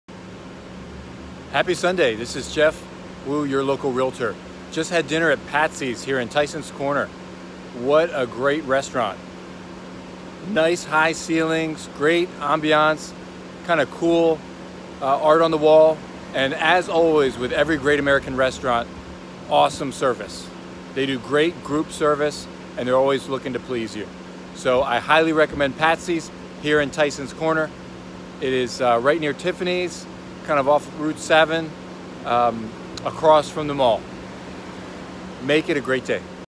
Patsys Tysons Corner a Great Place to eat
Patsys in Tysons Corner is a new restaurant by the local Great American Restaurant group.  Their menu has taken favorites from their other restaurants and put them all in one place.  Crabcake, ribs, lobster roll, come decide what your favorite is in this great new restaurant in Tysons Corner Virginia.